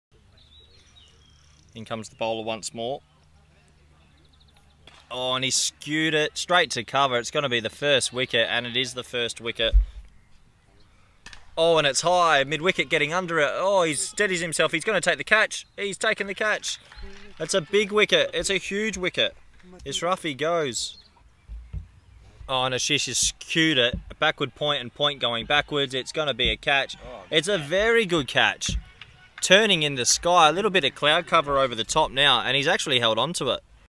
Three wicket burst Amazing Amin Zadran
Man of the Match: A. Zadran (3/7) - PSV vs SCE | Highlights | ECN Hannover Hitout, 2020 | European Cricket Series 2020